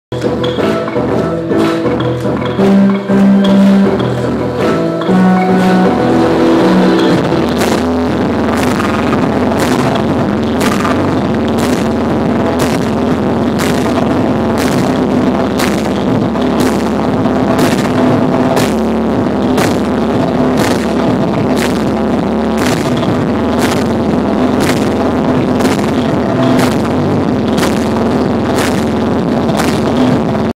Caribou, Live at LOLA Fest, September 18 2010
Backstage view of Caribou performing live.